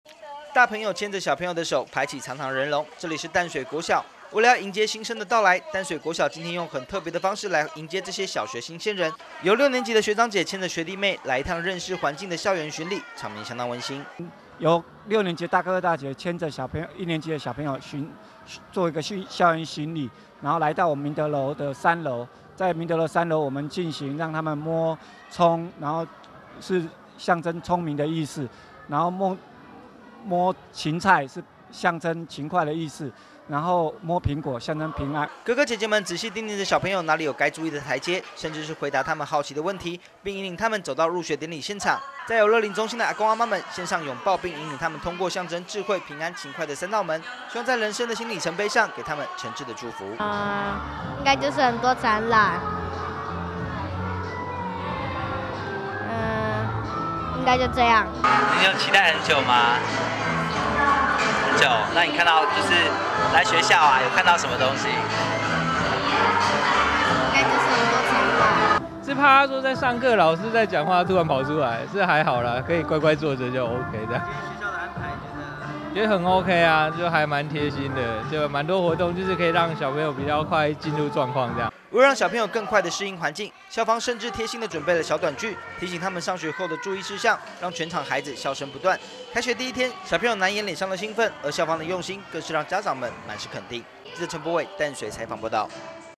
1030901紅樹林有線新聞 大手牽小手 淡小全校動員迎新生
九月一號到來，今天也是國中小學開學的日子，而新北市歷史最悠久的淡水國小，今天則是用很特別的方式，來歡迎新生，小一的新生，由六年級的大朋友牽著手，慢慢走進校園，帶他們認識即將要展開六年學習的新環境，接著新生的入學典禮則是由樂齡中心的阿公阿嬤們再給這些小朋友一個大擁抱和小禮物，場面相當溫馨。